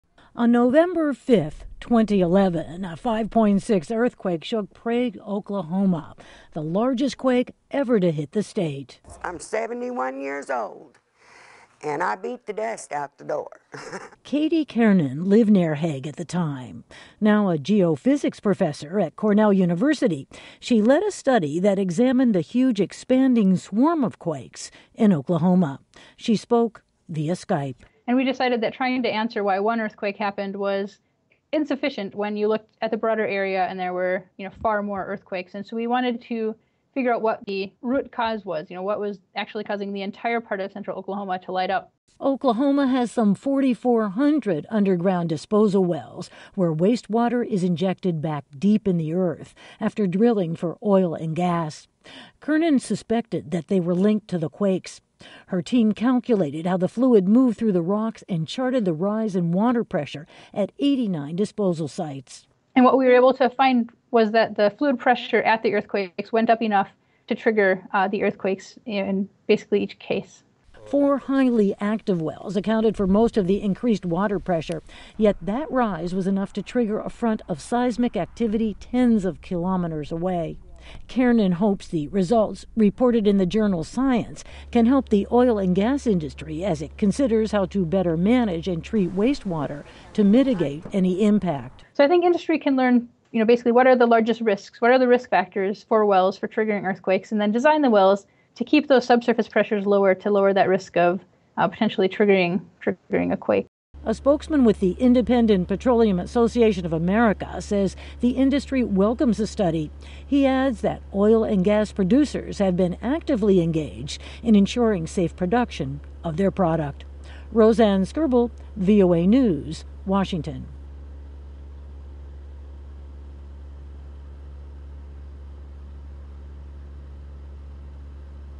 Seismic Surge Linked to Gas Industry Waste Wells in Oklahoma
Earthquakes are shaking the Midwestern state of Oklahoma, more than any other state in the country, even quake-prone California. Before 2009, Oklahoma had about two earthquakes of magnitude 3.0 or greater annually. So far this year, the number has skyrocketed to more than 200. As VOA's Rosanne Skirble reports, a new study finds clues to that increase in underground wells drilled by the gas and oil industry.